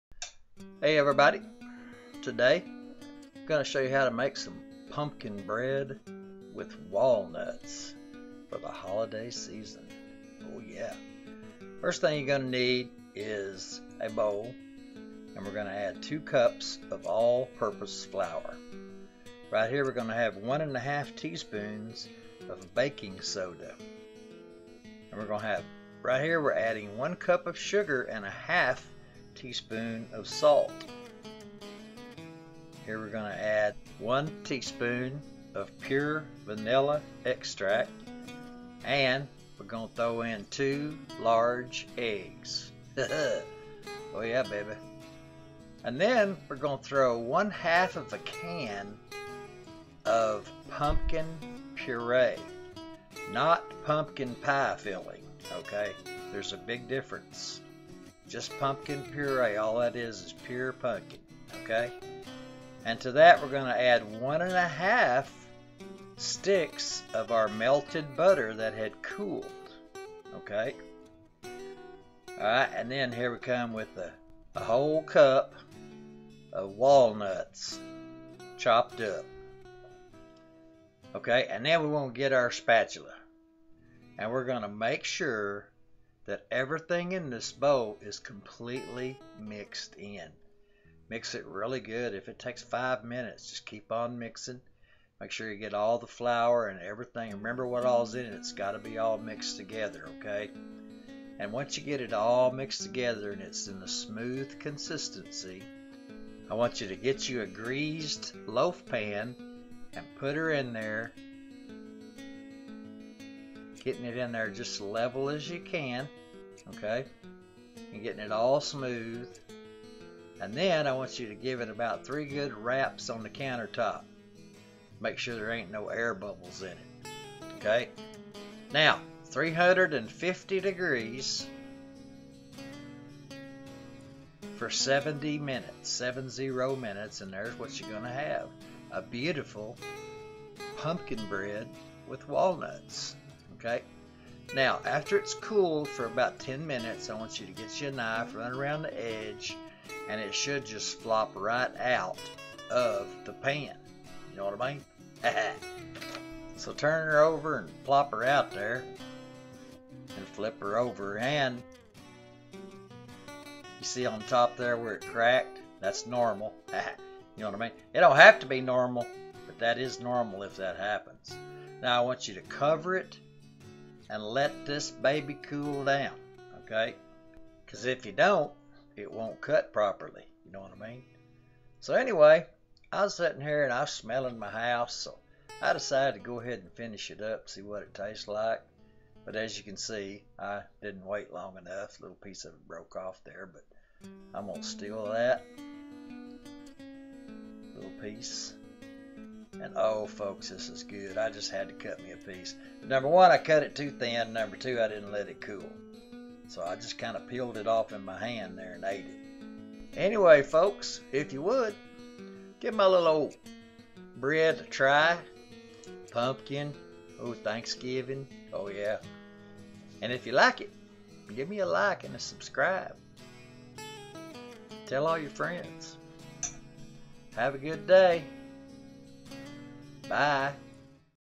Delicious Pumpkin Bread w/ Walnuts Holiday Bread
today we show you how to make our Pumpkin Bread
It is SO easy and delicious.. oh yea..
Ingredients
1/2 Can Pumpkin Puree (NOT Pumpkin Pie filling) 29 oz can
2 Cups all Purpose flour
1 1/2 teaspoons baking Soda
1 Cup Granulated Sugar
1/2 teaspoon Salt
1 1/2 sticks melted and COOLed Butter
1 Teaspoon Pure Vanilla
1 Cup Chopped Walnuts
Confectioners Sugar for Dusting..
Prepare as shown..